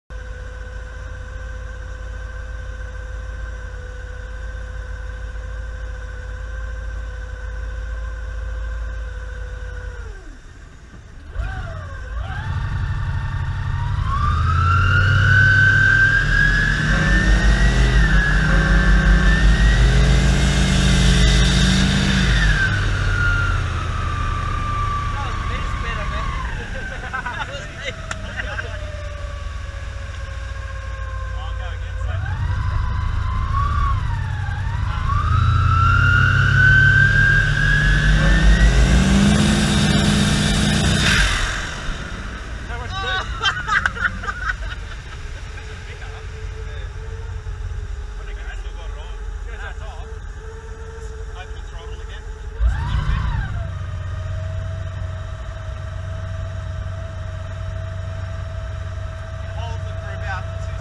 Turbo V8 Boat 10psi Run on Georges River
1st test run at 10psi after turbo install on Georges River